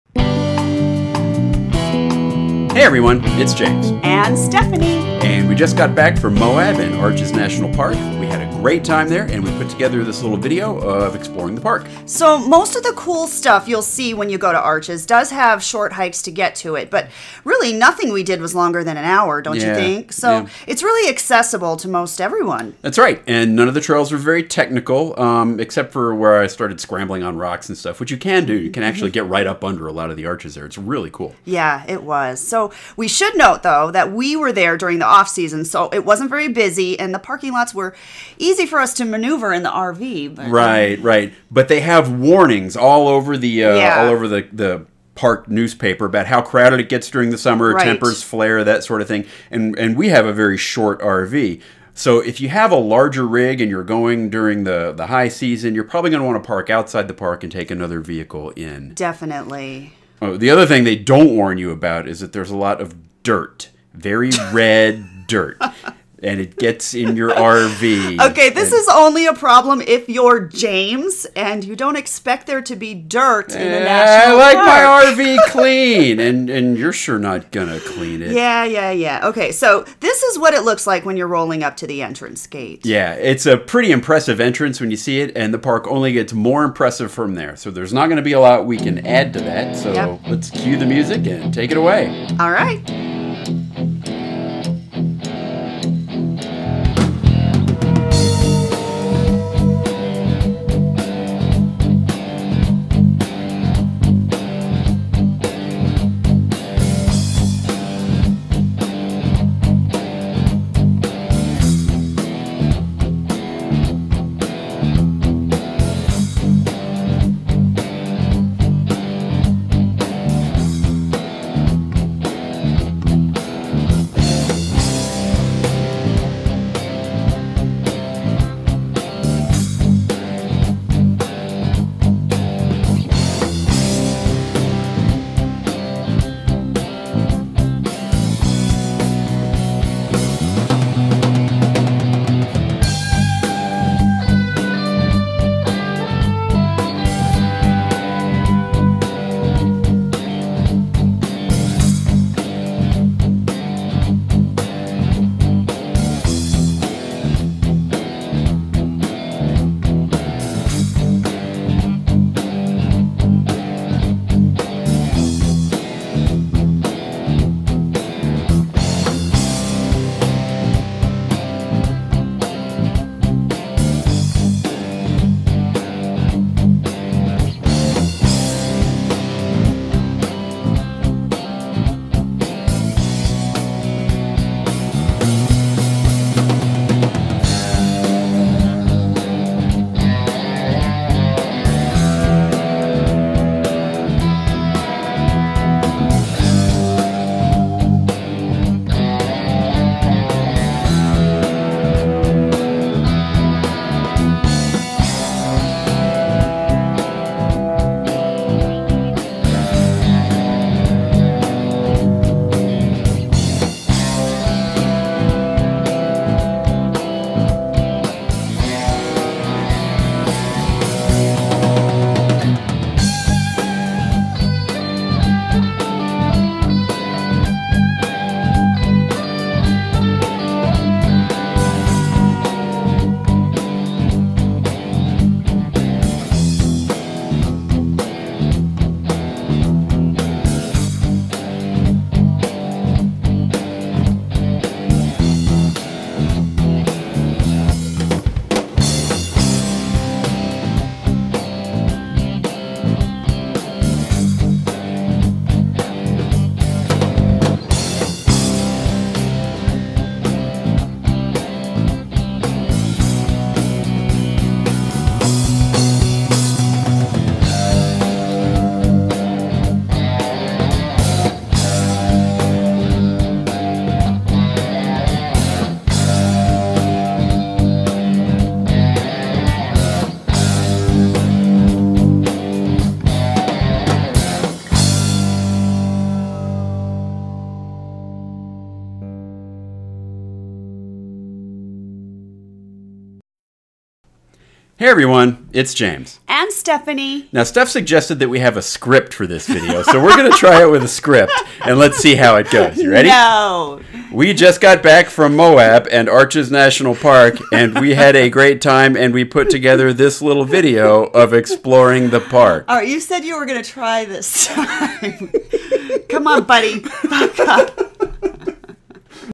Exploring Arches National Park
We recently took our RV down to Moab and Arches National Park.  Even though we were basically just sightseeing, we managed to get in some exercise with short hikes to the more popular arches and sites within the park.  There's also a bike path to the park that we rolled along at the beginning of the video.

You can't go wrong pointing your RV toward this beautiful area in Utah.  Enjoy the video!

- There's more than just the videos!